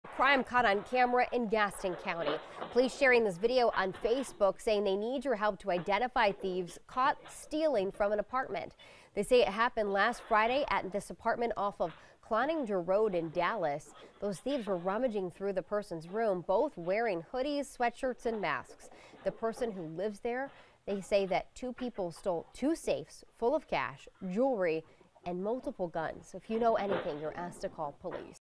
VIDEO: Surveillance Footage Shows Suspects Robbing Gaston Co. Apartment
Surveillance cameras captured two thieves stealing from a Gaston County apartment.